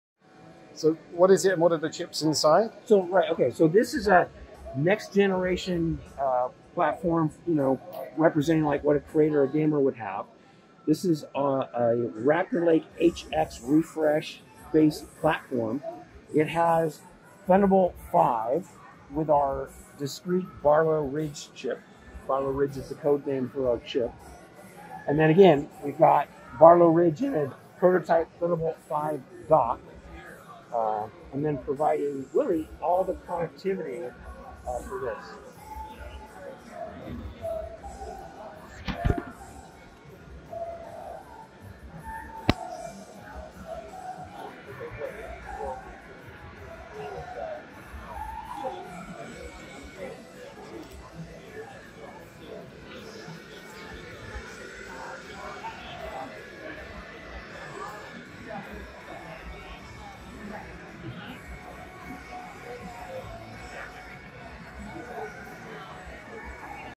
Gigabyte Aorus 17 Thunderbolt 5 laptop unveiled by Intel
At Intel's "Intel Innovation" Developer Conference in San Jose, we saw one of the very first laptops with a discreet Thunderbolt 5 "Barlow Ridge" chip - a 13th Gen Gigabyte Aorus 17x. We're told we should start seeing more Thunderbolt 5 laptops appearing in 2024.